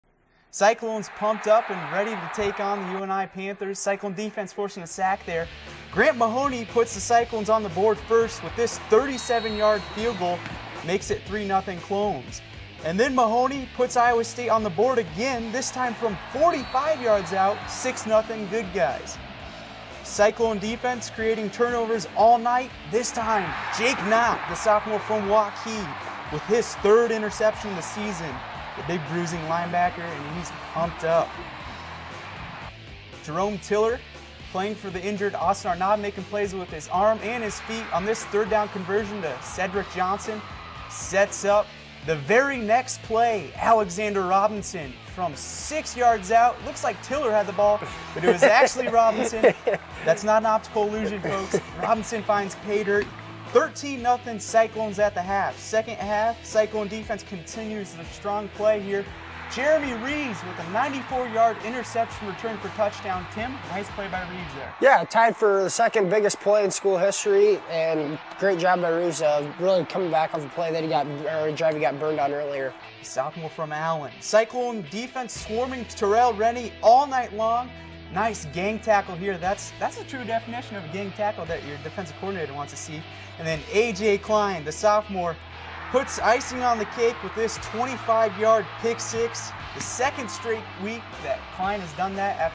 ISU v UNI Highlights
Cyclone Football Weekly breaks down plays from the ISU v UNI game.